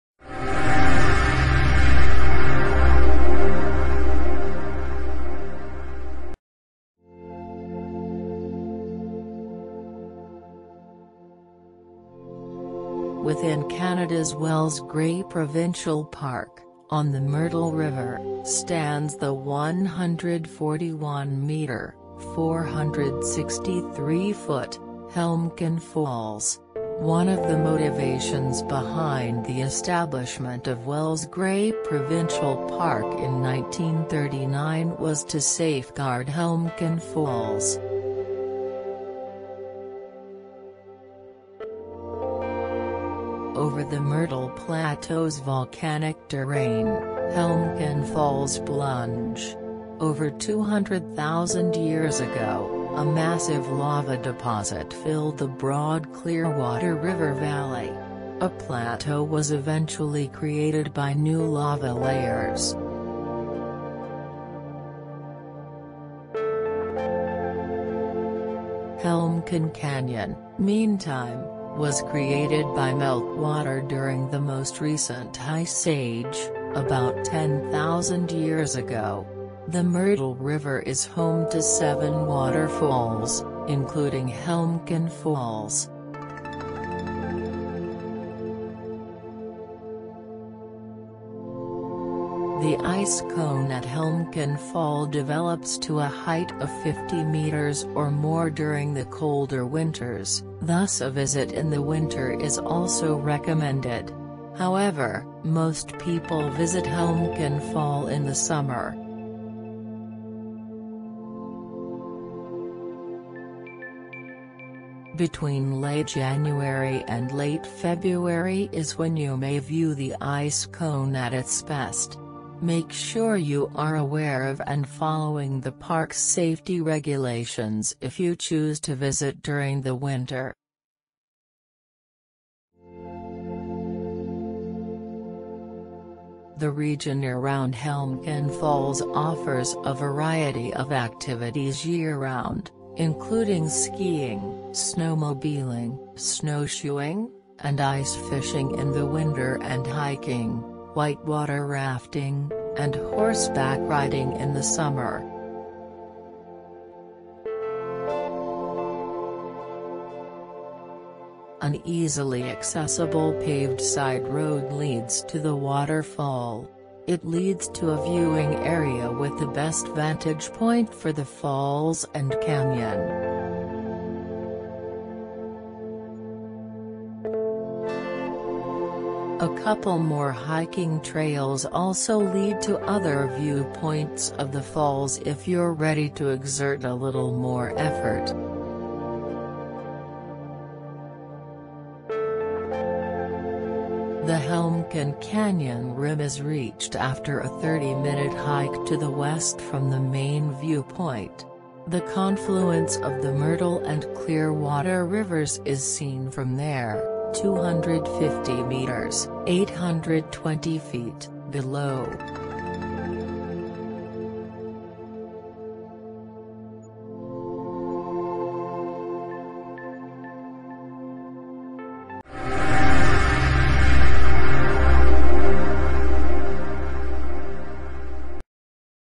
Helmcken Waterfall (Canada)
One of the motivations behind the establishment of Wells Gray Provincial Park in 1939 was to safeguard Helmcken Falls.